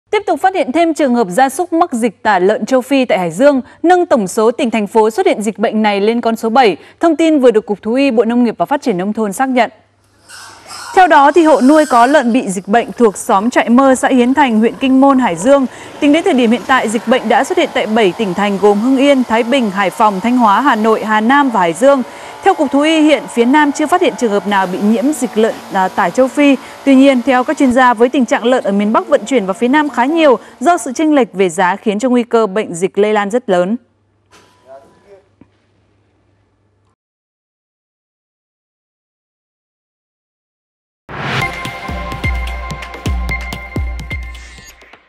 Dịch tả lợn Châu Phi đã lan ra 7 tỉnh thành, trong đó có Hải Dương | VTV24
Tiếp tục phát hiện thêm trường hợp gia súc mắc dịch tả lợn châu Phi tại Hải Dương, nâng tổng số tỉnh, thành phố xuất hiện dịch bệnh này lên con số 7.